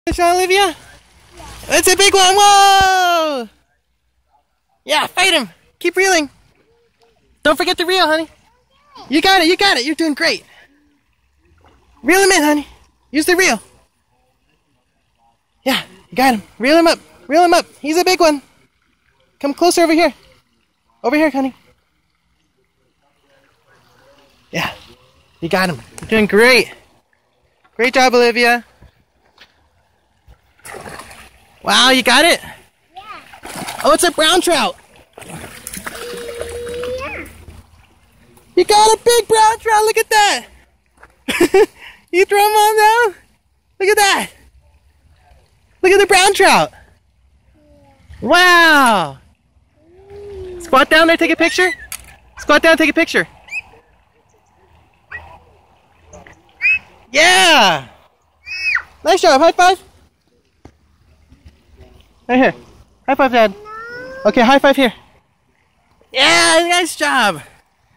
Olivia landing a brownie
Olivia catching a 15 inch brown at Tahoe Trout Farm.